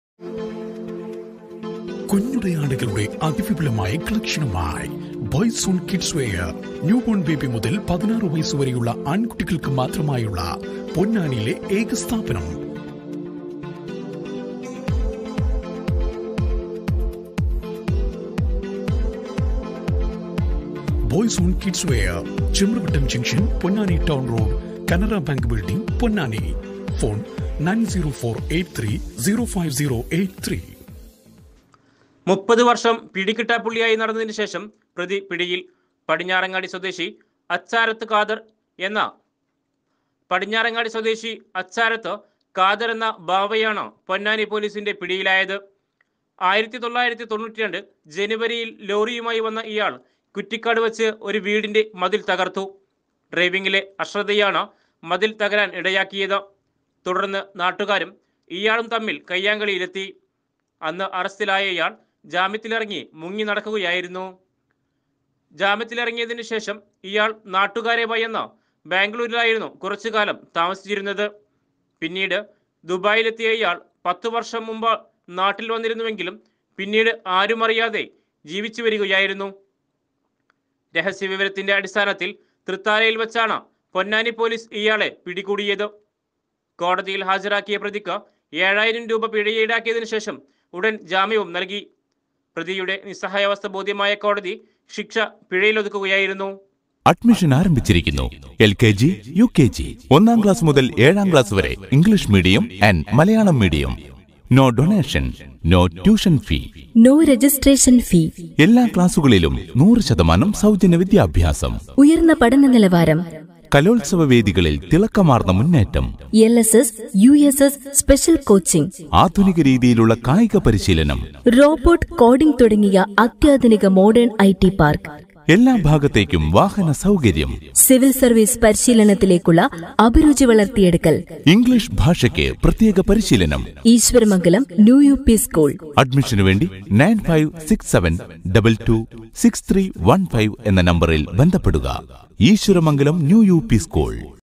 30 വർഷം പിടികിട്ടാപുള്ളി.ടുവിൽ പിടിയിൽ
30 വർഷം പിടികിട്ടാപുള്ളി.
ഒടുവിൽ പിടിയിൽ

സഹതാപം തോന്നിയ കോടതി പിഴ ചുമത്തി വെറുതെ വിട്ടു.

പൊന്നാനി: 30 വർഷം പിടികിട്ടാപ്പുള്ളിയായി നടന്നതിന് ശേഷം പ്രതി പിടിയിൽ. പടിഞ്ഞാറങ്ങാടി സ്വദേശി അച്ചാരത്ത് കാദർ എന്ന ബാവയാണ് പൊന്നാനി പോലീസിൻ്റെ പിടിയിലായത്.

1992 ജനുവരിയിൽ ലോറിയുമായി വന്ന ഇയാൾ കുറ്റിക്കാട് വെച്ച് ഒരു വീടിൻ്റെ മതിൽ തകർത്തു. ഡ്രൈവിംഗിലെ അശ്രദ്ധയാണ് മതിൽ തകരാൻ ഇടയാക്കിയത്.തുടർന്ന് നാട്ടുകാരും ഇയാളും തമ്മിൽ കൈയ്യങ്കാളിയിലെത്തി. അന്ന് അറസ്റ്റിലായ ഇയാൾ ജാമ്യത്തിലിറങ്ങി മുങ്ങി നടക്കുകയായിരുന്നു. ജാമ്യത്തിലിറങ്ങിയതിന് ശേഷം ഇയാൾ നാട്ടുകാരെ ഭയന്ന് ബാംഗ്ലൂരിലായിരുന്നു കുറച്ചു കാലം താമസിച്ചിരുന്നത്.പിന്നീട് ദുബായിലെത്തിയ ഇയാൾ 10 വർഷം മുമ്പ് നാട്ടിൽ വന്നിരുന്നുവെങ്കിലും പിന്നീട് ആരുമറിയാതെ ജീവിച്ചുവരികയായിരുന്നു.

രഹസ്യവിവരത്തിൻ്റെ അടിസ്ഥാനത്തിൽ തൃത്താലയിൽ വെച്ചാണ് പൊന്നാനി പോലീസ് ഇയാളെ പിടികൂടിയത്. കോടതിയിൽ ഹാജരാക്കിയ പ്രതിക്ക് ഏഴായിരം രൂപ പിഴയിട്ടതിന് ശേഷം ഉടൻ ജാമ്യവും നൽകി.പ്രതിയുടെ നിസ്സഹായവസ്ഥ ബോധ്യമായ കോടതി ശിക്ഷ പിഴയിലൊതുക്കുകയായിരുന്നു.